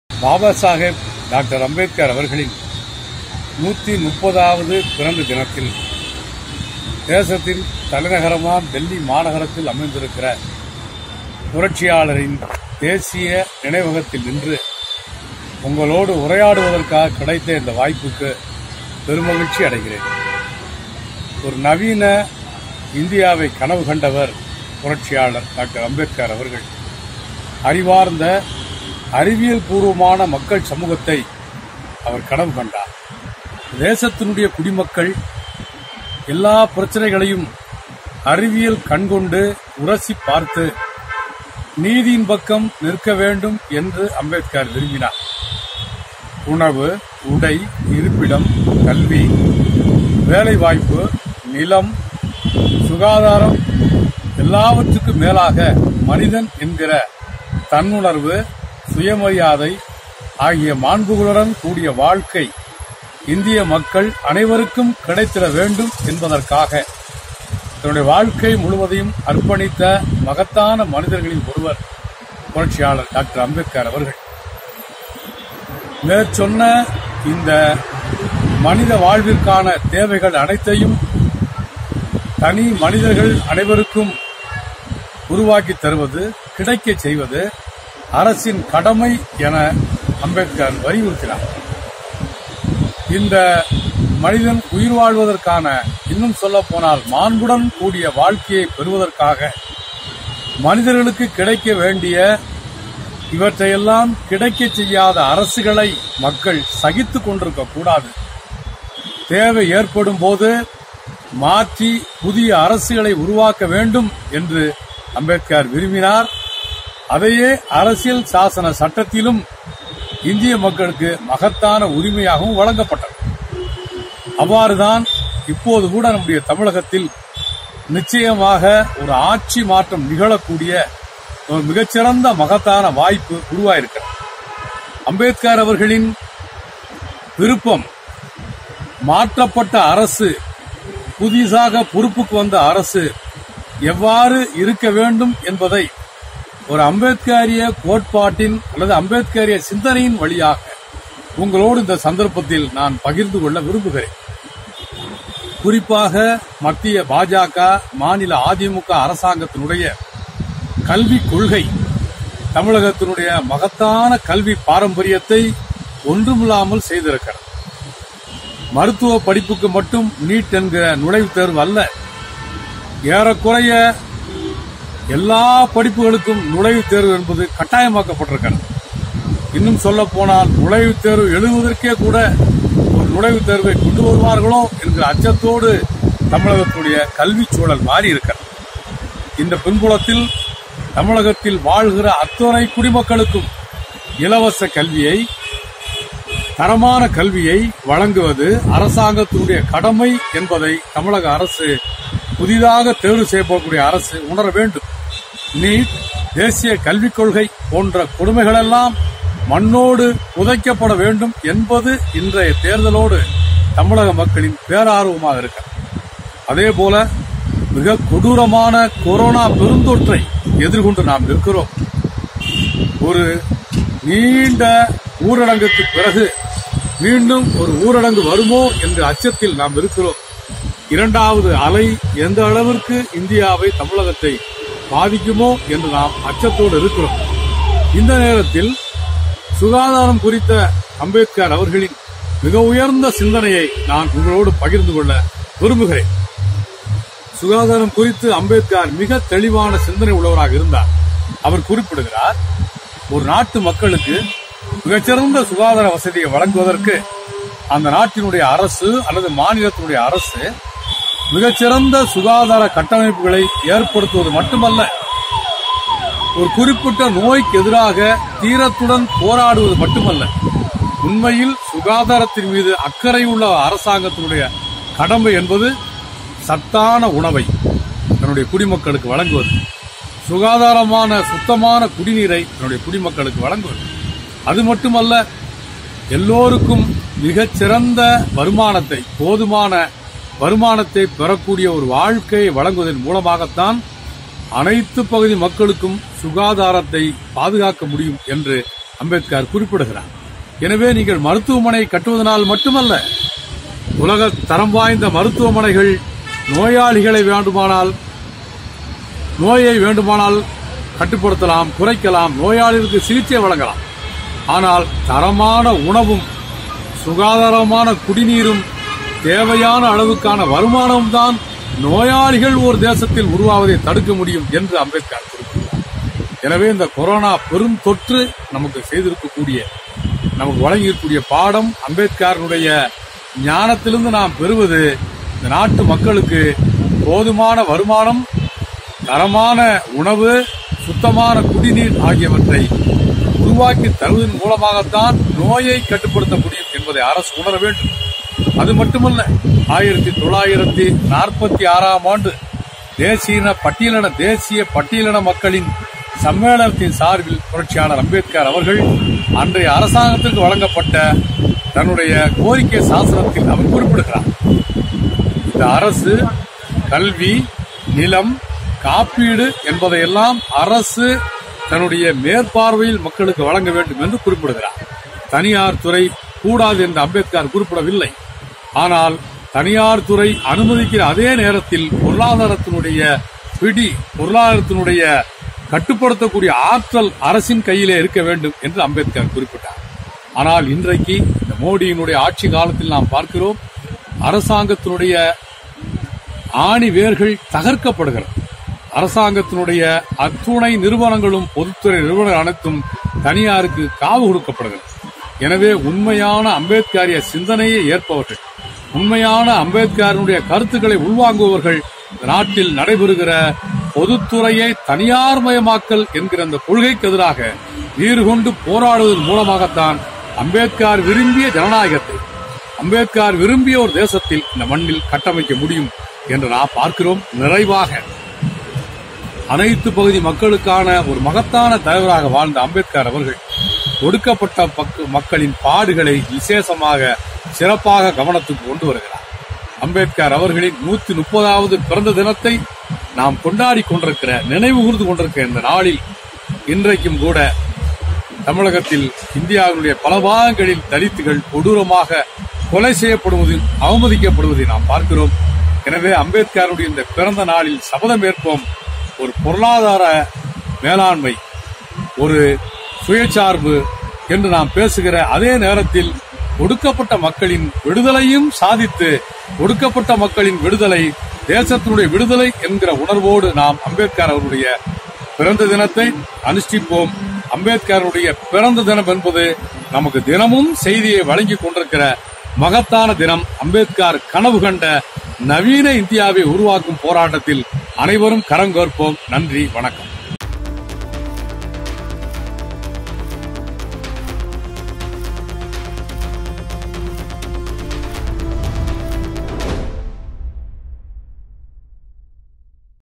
அண்ணலின் கனவு மெய்ப்பட நாம் என்ன செய்யவேண்டும்? - தோழர் சாமுவேல் ராஜ் | AmbedkarJayanti | CPIM
அம்பேத்கர் அவர்களின் 130ஆம் பிறந்த நாளில் அவர் கனவு கண்ட நவீன இந்தியாவை உருவாக்க நாம் என்ன செய்யவேண்டும் என்று தெளிவுப்படுத்திகிறார் தோழர் சாமுவேல் ராஜ்.